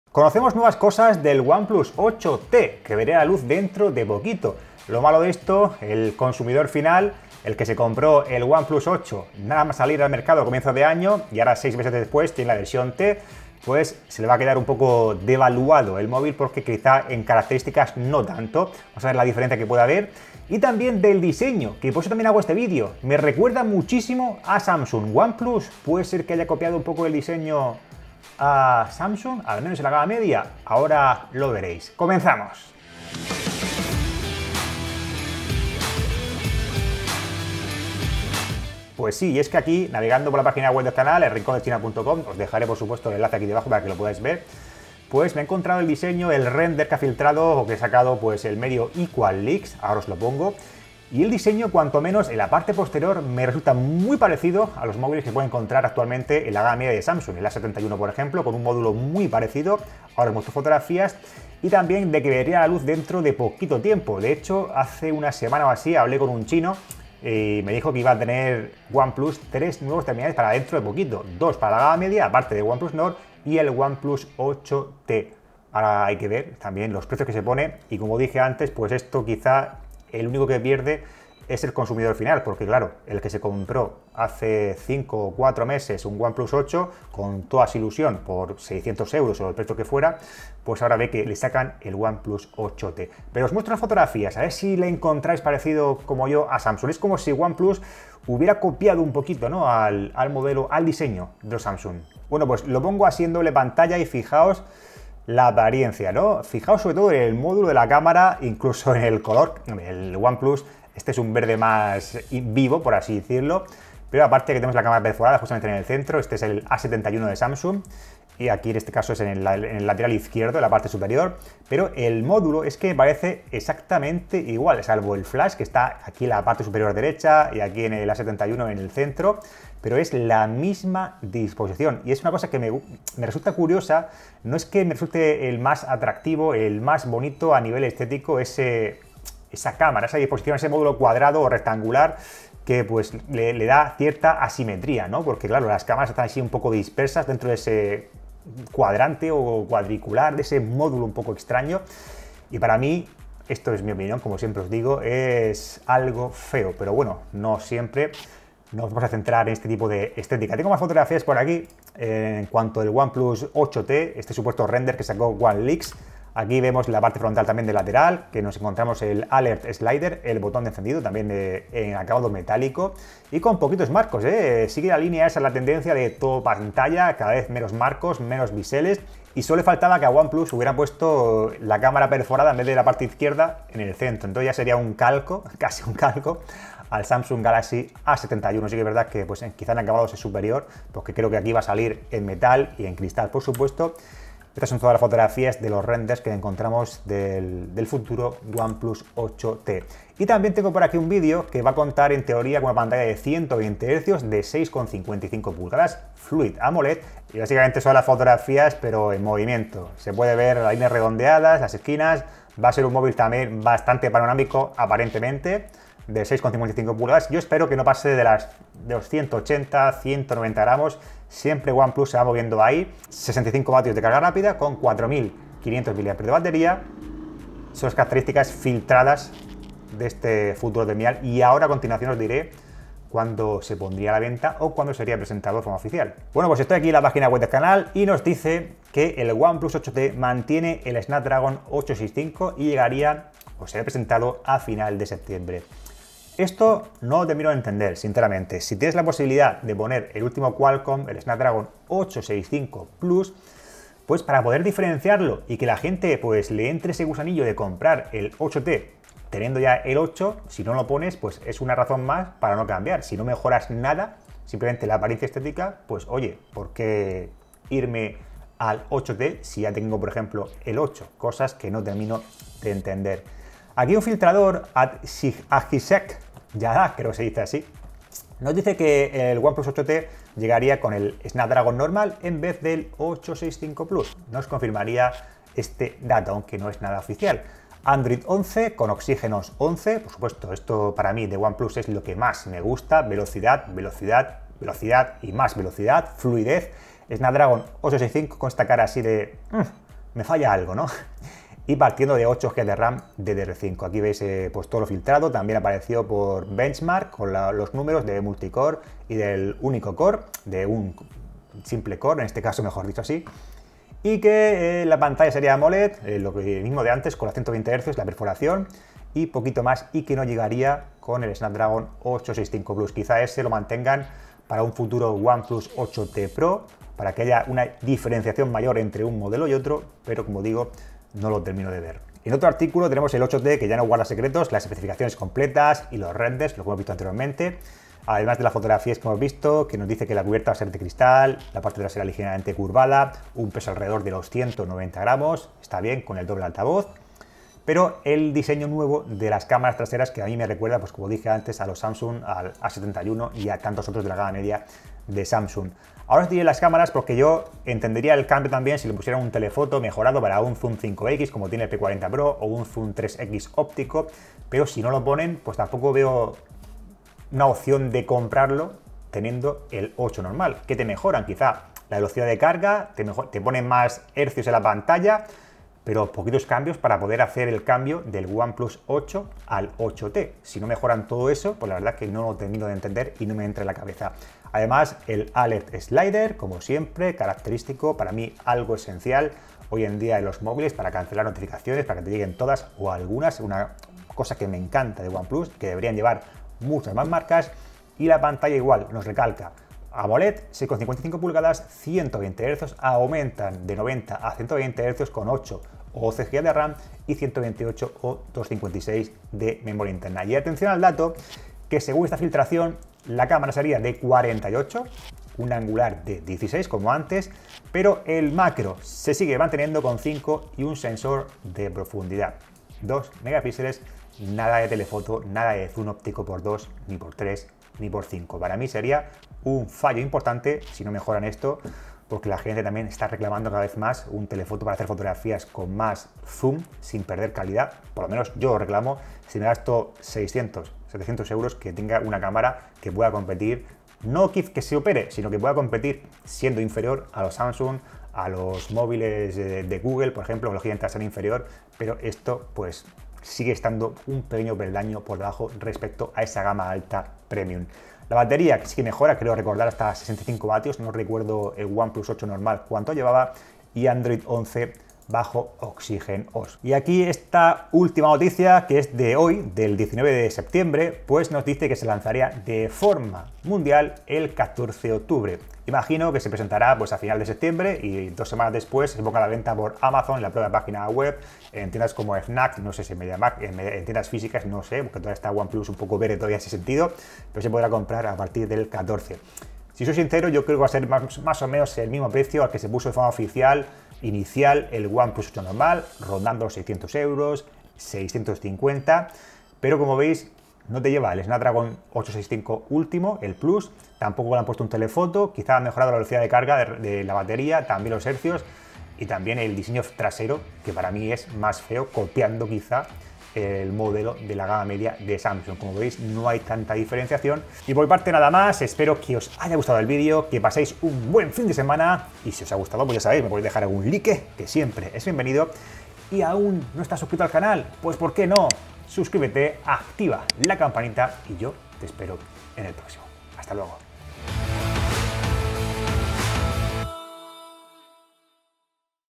Oneplus COPIA a SAMSUNG con su Oneplus 8T ¿O me lo parece?
Bienvenidos de nuevo a El Rincón de China !!!

Hoy os cuento todo lo que se sabe del próximo lanzamiento de Oneplus para su gama alta, el 8T.

¿Mejorará tanto como para plantearse el cambio del 8 "normal" a esta versión "T"? Lo vemos en este vídeo.